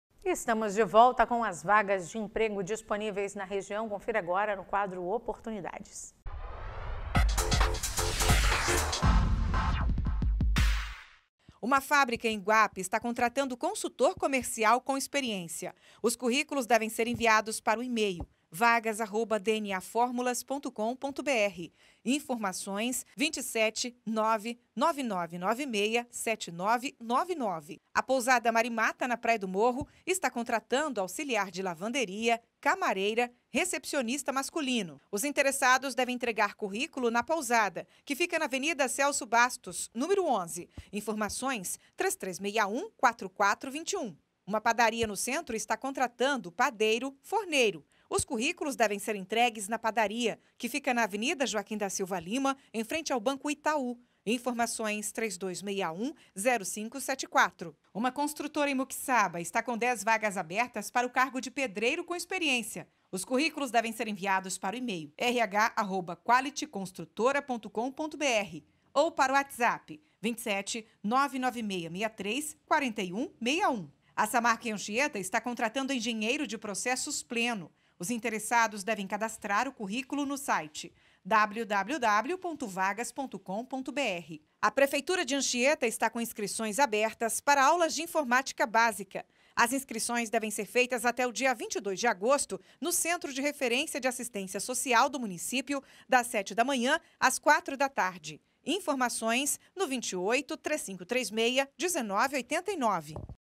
17/08/2022 - OPORTUNIDADES
Estamos de volta com as vagas de emprego disponíveis na região. Veja no quadro Oportunidades.